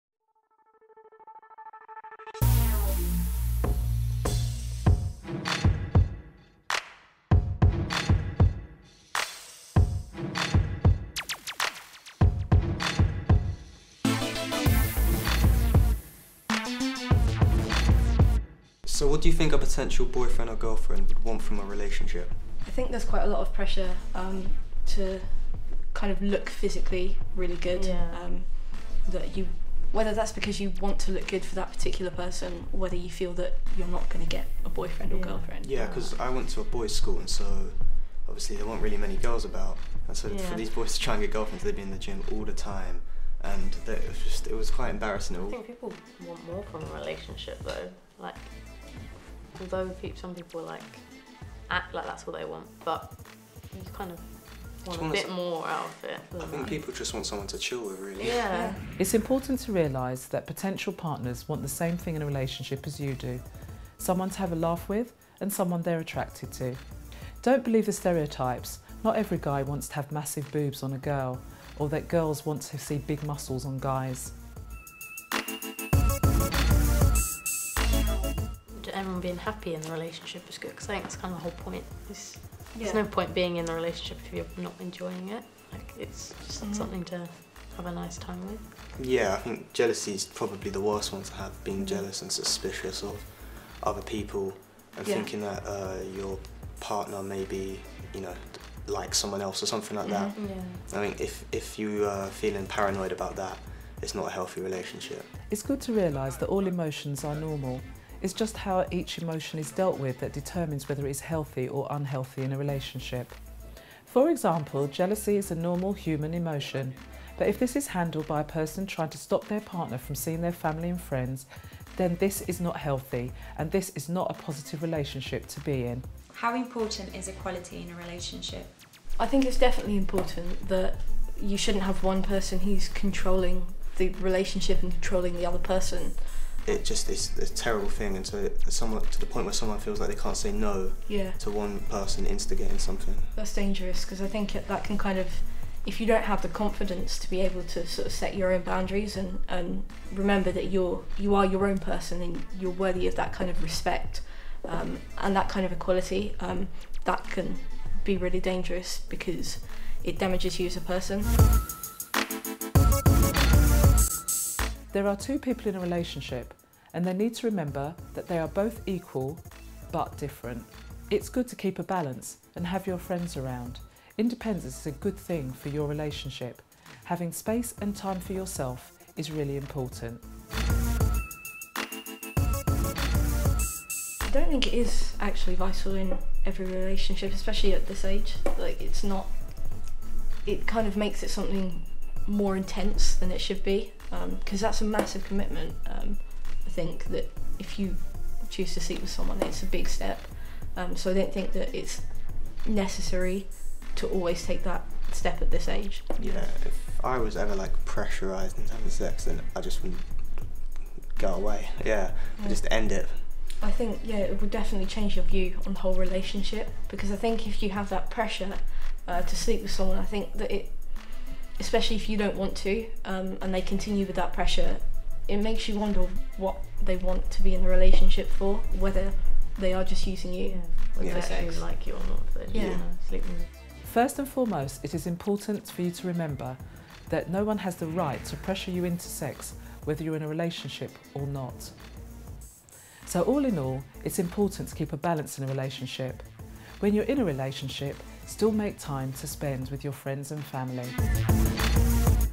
Healthy relationships
A video made by young people discussing what to expect from relationships, what are healthy relationships and what are unhealthy.